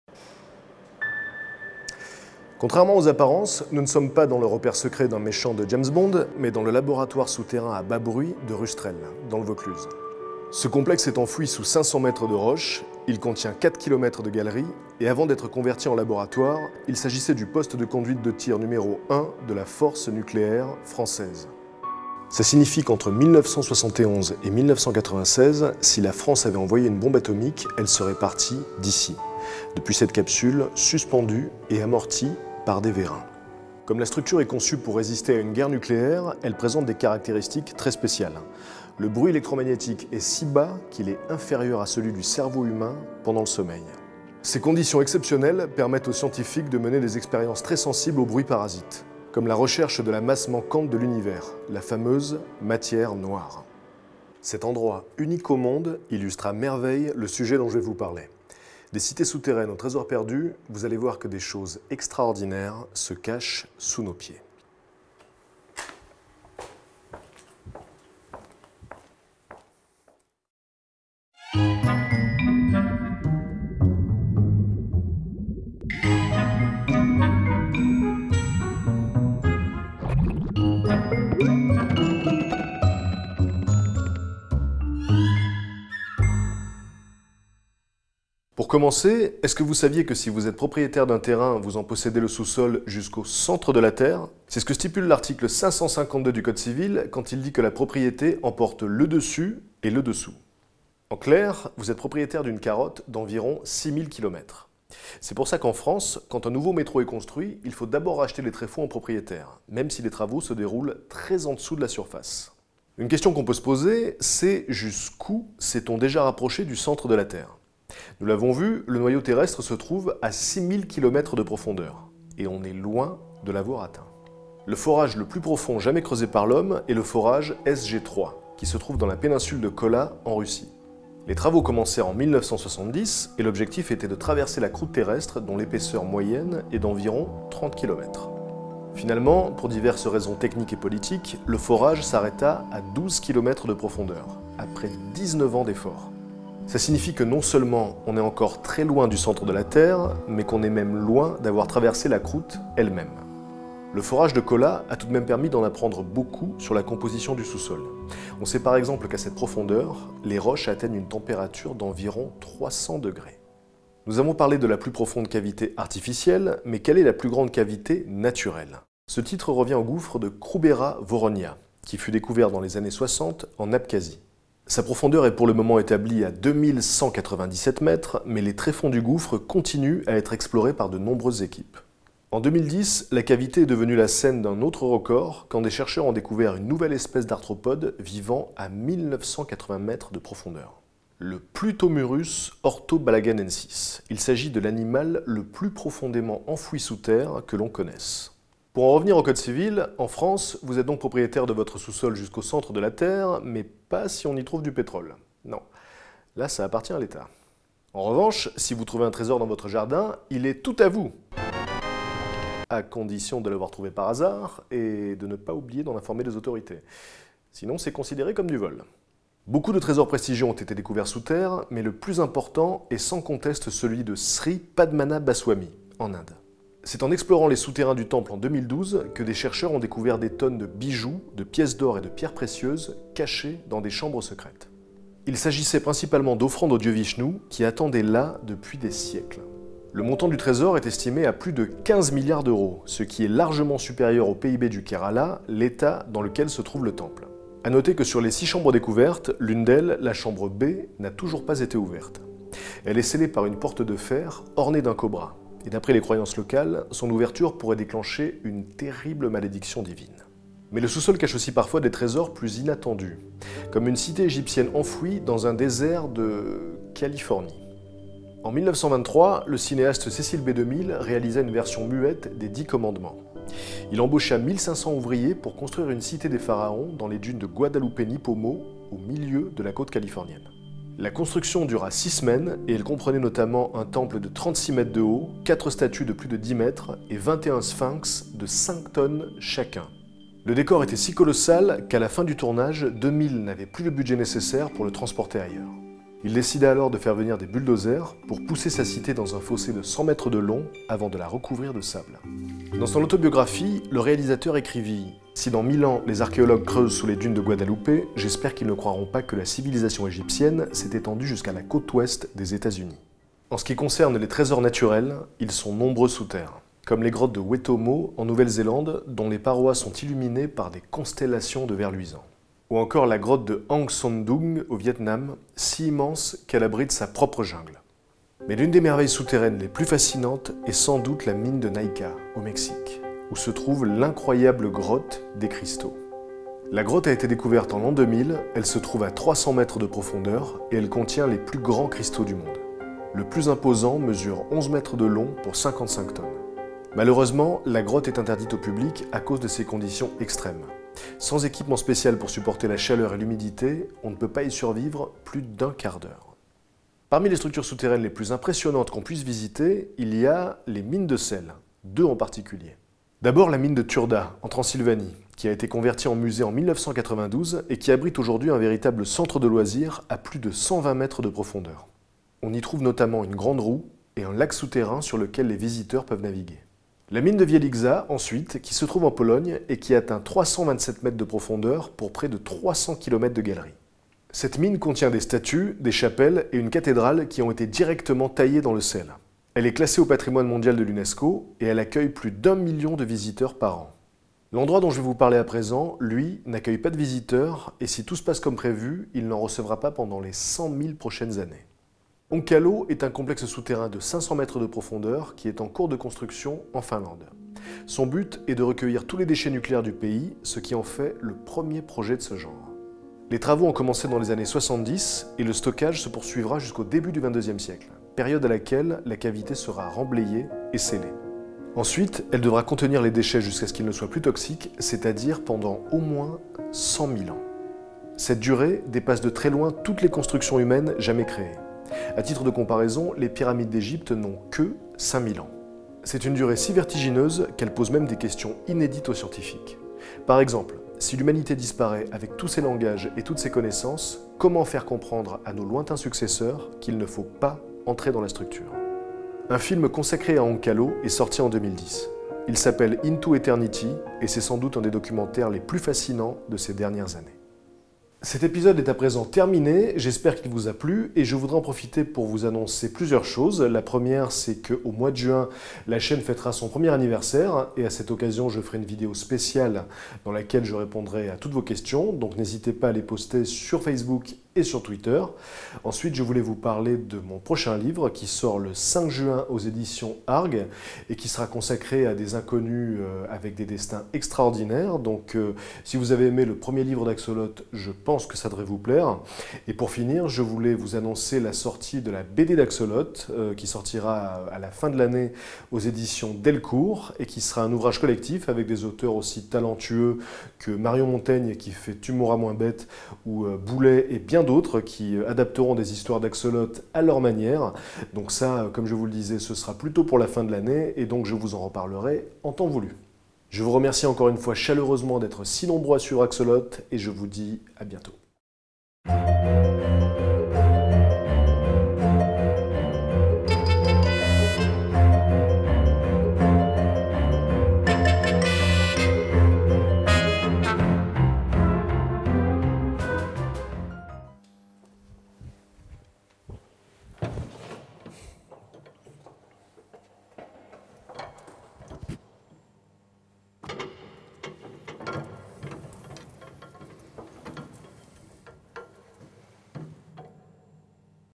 Axolot #7 : Sous Terre
Ecrit et présenté par : Patrick Baud
Réalisation : Elsa Kmiec, Patrick Baud
Générique : Thomas Lecourt

Merci au CNRS et à Alain du laboratoire souterrain à bas bruit de Rustrel pour nous avoir accueilli avec autant de patience et de gentillesse !

Merci encore à Gilles et Vincent de chez Image Mouvement pour avoir assuré les prises de vue et les prises son de l'intro, ces gars sont furieusement cool et font du bon boulot